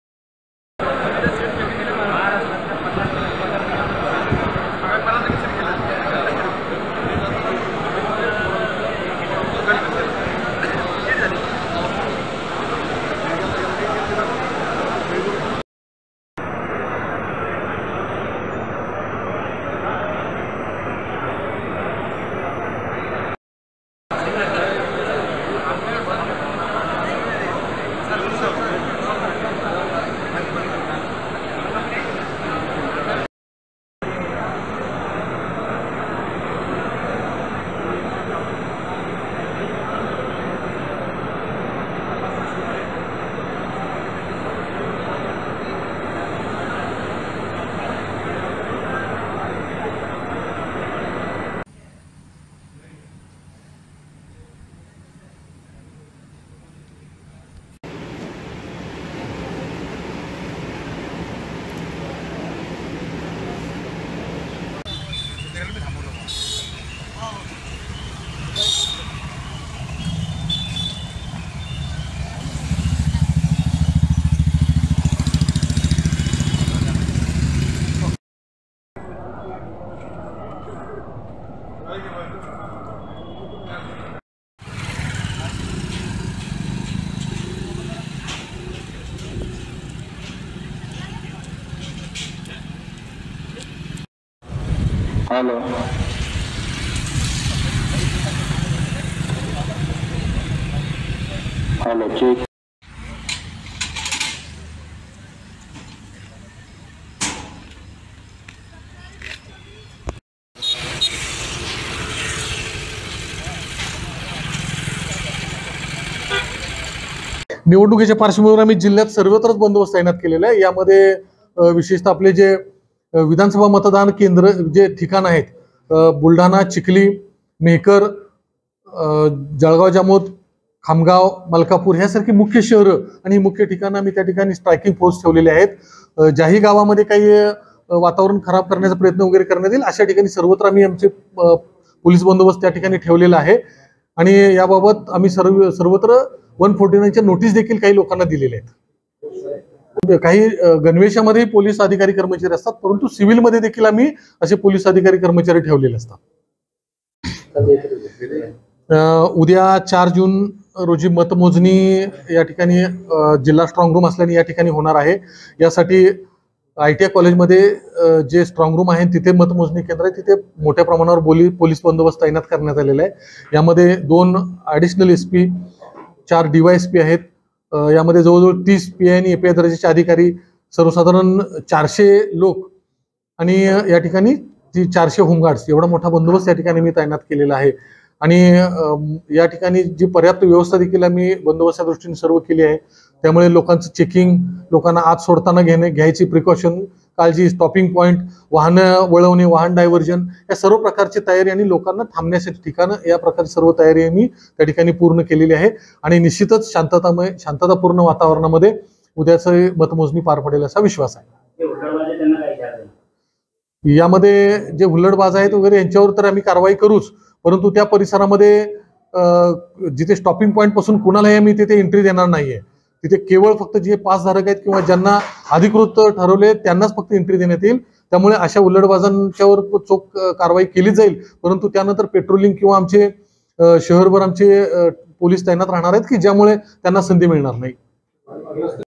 BULDHANA | लोकसभा निवडणूक मतमोजणीसाठी पोलीस यंत्रणा सज्ज, केंद्रा बाहेर पोलिसांचा कडेकोट बंदोबस्त
बुलढाणा लोकसभा मतदार संघाची मतमोजणी मंगळवारी बुलढाणा औद्योगिक संस्थेत होणार आहे. मतमोजणीदरम्यान कोणताही अनुचित प्रकार घडू नये यासाठी पोलीस विभागाकडून कडेकोट पोलीस बंदोबस्त करण्यात आला आहे. यावेळी परवानगी आणि प्राधिकार पत्र बाळगणाऱ्यांनाच मतमोजणी केंद्रात प्रवेश दिला जाणार आहे. मतमोजणीदरम्यान कायदा व सुव्यवस्था आबाधित राहावी, यासाठी दोन अतिरिक्त पोलिस अधीक्षक, ४ डीवायएसपी, ३० पोलिस निरीक्षक अणि सहाय्यक निरीक्षक, ४०० पोलिस कर्मचारी आणि ४०० होमगार्ड बंदोबस्त ठेवण्यात आला आहे. मतमोजणी केंद्रात मोबाईल, लॅपटाप नेण्यास बंदी आहे. मतमोजणी केंद्रात ओळखपत्राशिवाय जाता येणार नाही, याशिवाय १४९ च्या नोटीस देखील काही लोकांना देण्यात आली आहे. अशी माहिती आज अप्पर जिल्हा पोलीस अधीक्षक बीबी महामुनी यांनी दिली आहे. तसेच जिल्ह्यातील संवेदनशील ठिकाणी पोलिस बंदोबस्त देखील ठेवण्यात आला आहे.

BYTE - बी.बी महामुनी, अप्पर जिल्हा पोलीस अधीक्षक बुलढाणा.